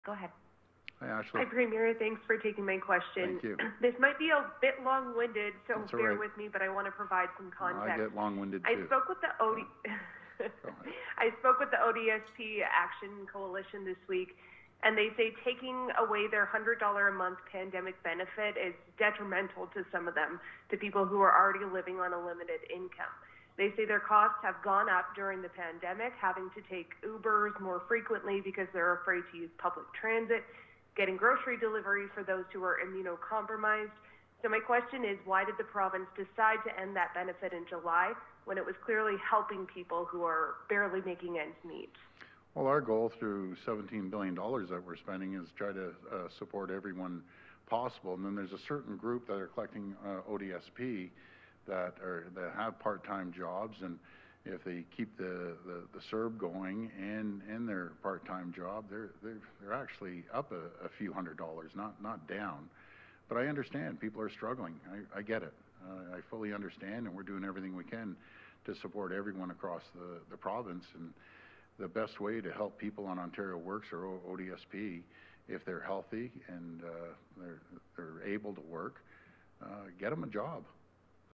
Doug Ford Tell's Disabled People on ODSP to get a Job or Based on Current Actions Suffer & Die...
Once again Doug Ford Dislikes Disabled People especially those on ODSP who are in need. His attack once again is get a job or Die. This Die part is their actions on those who can't work and get no extra help. If Doug Ford and this Government are not helping more lives will be lost in the long run.

People on ODSP already live almost 50% under the poverty Line. The System exist for those who can't work or not healthy enough to have a job in the first place.